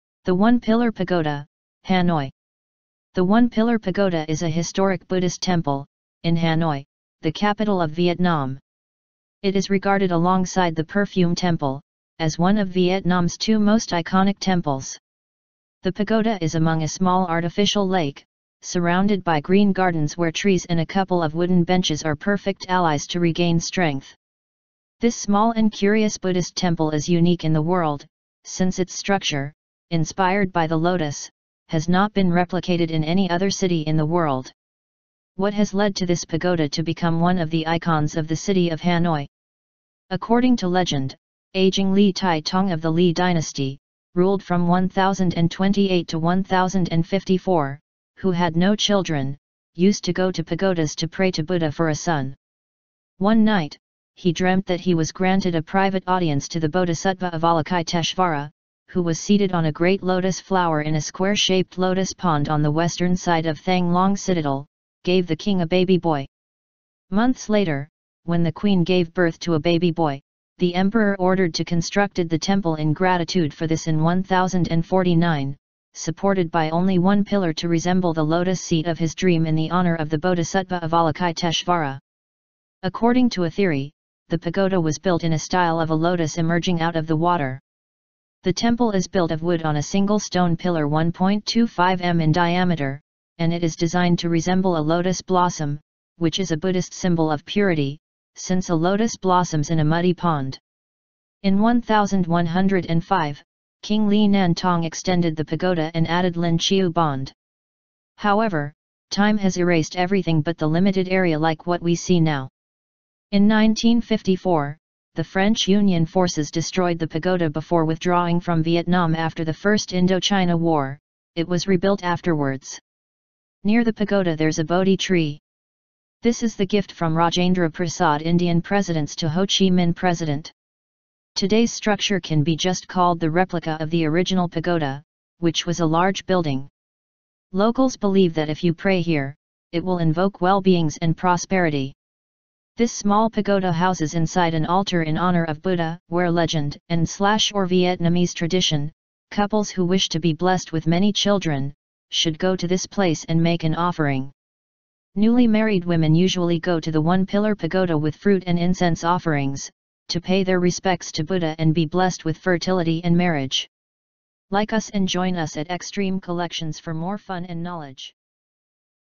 The One Pillar Pagoda, Hanoi
The One Pillar Pagoda is a historic Buddhist temple, in Hanoi, the capital of Vietnam. It is regarded alongside the Perfume Temple, as one of Vietnam's two most iconic temples. The Pagoda is among a small artificial lake, surrounded by green gardens where trees and a couple of wooden benches are perfect allies to regain strength. This small and curious Buddhist temple is unique in the world, since its structure, inspired by the Lotus, has not been replicated in any other city in the world. What has led to this Pagoda to become one of the icons of the city of Hanoi.
According to legend, ageing Lý Thái Tông of the Ly dynasty, ruled from 1028 to 1054, who had no children, used to go to pagodas to pray to Buddha for a son. One night, he dreamt that he was granted a private audience to the Bodhisattva Avalokiteshvara, who was seated on a great lotus flower in a square-shaped lotus pond on the western side of Thang Long Citadel, gave the King a baby boy. Months later, when the Queen gave birth to a baby boy, The emperor ordered to constructed the temple in gratitude for this in 1049, supported by only one pillar to resemble the lotus seat of his dream in the honor of the Bodhisattva Avalokiteshvara. According to a theory, the pagoda was built in a style of a lotus emerging out of the water. The temple is built of wood on a single stone pillar 1.25 m in diameter, and it is designed to resemble a lotus blossom, which is a Buddhist symbol of purity, since a lotus blossoms in a muddy pond.
In 1105, King Ly Nhan Tong extended the pagoda and added Linh Chieu bond. However, time has erased everything but the limited area like what we see now. In 1954, the French Union forces destroyed the pagoda before withdrawing from Vietnam after the First Indochina War, It was rebuilt afterwards. Near the pagoda there’s a bodhi tree. This is the gift from Rajendra Prasad- Indian presidents to Ho Chi Minh President. Today's structure can be just called the replica of the original pagoda, which was a large building. Locals believe that if you pray here, it will invoke well-beings and prosperity.
This small pagoda houses inside an altar in honor of Buddha, where legend and / or Vietnamese tradition, couples who wish to be blessed with many children, should go to this place and make an offering. Newly married women usually go to the One Pillar Pagoda with fruit and incense offerings, to pay their respects to Buddha and be blessed with fertility and marriage.